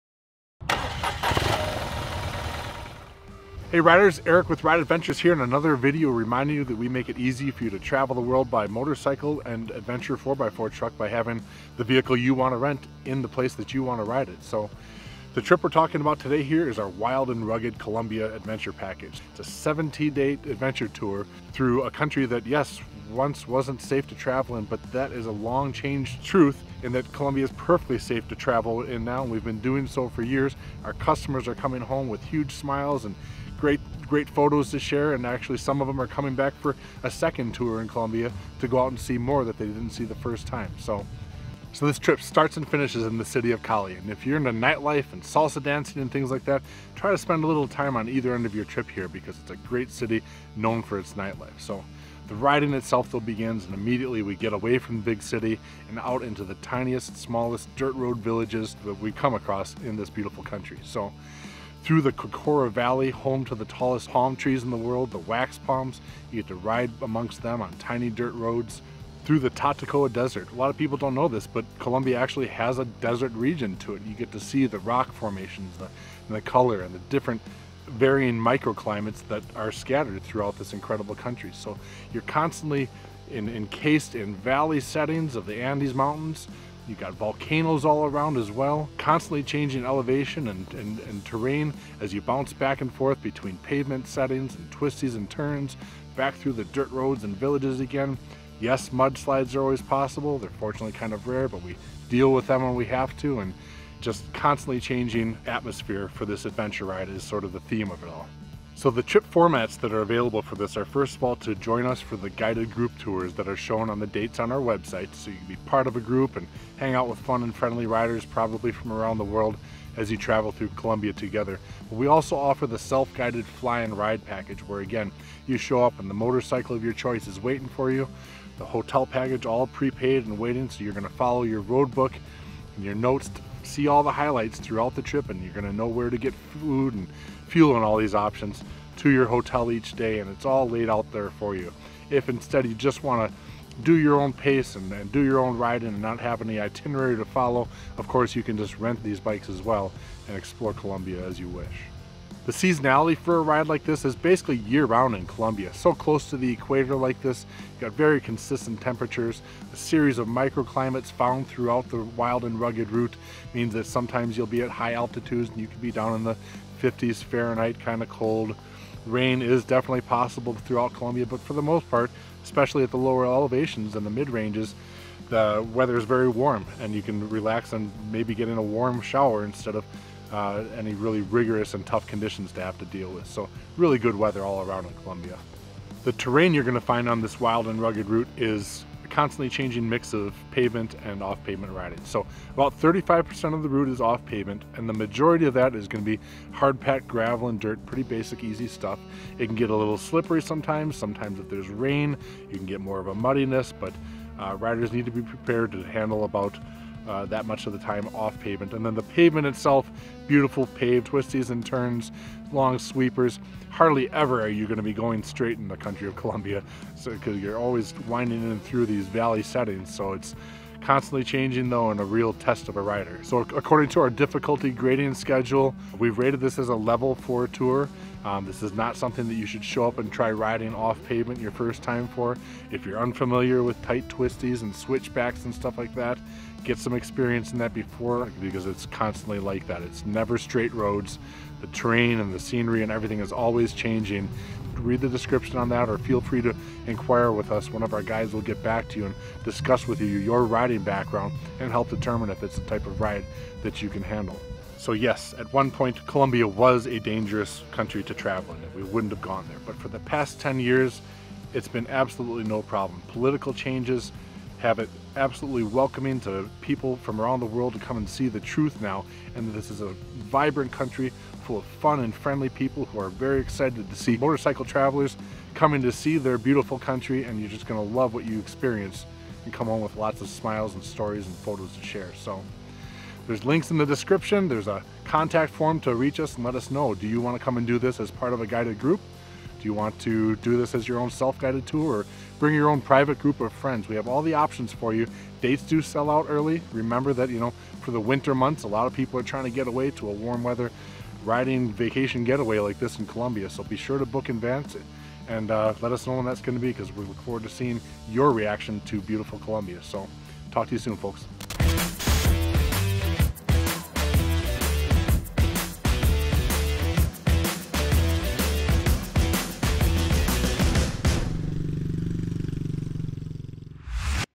Our Wild and Rugged Adventure Motorcycle Tour in Colombia | What's It Really Like?
🏍️We're an adventure motorcycle tour company making it easy for you to fly and ride the bike of your choice in the country you want. In this video, we show you what it's really like to take one of our adventure motorcycle tours in Colombia!  From the outstanding views of the Cocora Valley, Tatacoa Desert, and coffee plantation region, through the gorgeous countryside full of twisty roads, both on pavement and off, this Wild and Rugged itinerary is designed to give riders true insight to Colombian life, away from major cities as much as possible.

🙌Colombia Adventure Motorcycle Tour stats

Origin: Cali, Colombia (CLO Airport)
Destination: Cali, Colombia
Dates of tour: Please see "Dates" tab
Trip Duration: 17 Days (1 Arrival, 1 Departure, 13 Riding Days and 2 rest days.)
Distance: 3125 kilometers (1942 miles)
Average ride per day: 4-7 hours on the bike (240 km / 149 miles per day - Average, but large differences in mileage on this trip)
Terrain: 35% dirt/gravel roads, 65% pavement
Weather: Variable depending elevation, and location. Typically warm or hot, but cooler at high elevation passes. So close to the equator, trips usually range from 10C/50F at high elevations to 35C/95F when down in valleys. Summer/vented gear is recommended with light liner and rain gear.

🙌Highlights for the "Wild and Rugged" Colombia Motorcycle tour

-COCORA VALLEY, the origins of the Quindío River and home to the Wax Palms, the world's tallest palm trees welcome both riding and hiking possibilities

-THE TOWN OF JARDIN, with its world-famous Colombian coffee and plantain fields

-STONE OF EL PEÑOL is a landmark of Colombia providing incredible views. This massive rock formation is millions of years old, and you'll be blown away by the 740-step climb to the top!

-CHICAMOCHA CANYON where you can enjoy the stunning scenery on your RIDE, or choose the adventurous way of paragliding

-TATACOA DESERT - You'll need to go on a mini side adventure to experience this destination oasis, crossing bridges, riding through tunnels, and crossing the Magdalena River in a canoe just to get there

-SAN AGUSTÍN - Considered the world's largest necropolis and an UNESCO World Heritage site, the area is full of religious monuments and megalithic sculptures. Great rest day activities are possible here for the important off-bike experiences.

-THE ANDES MOUNTAINS, where you can enjoy the stunning scenery on your RIDE. We've ridden all over South America, and can't emphasize this enough: You'll never forget the majesty of the Andes!

Thanks to Richard for a few clips on the nightlife in Cali. If you guys want to see more of what's it like in Cali during the night check out this guy's YouTube at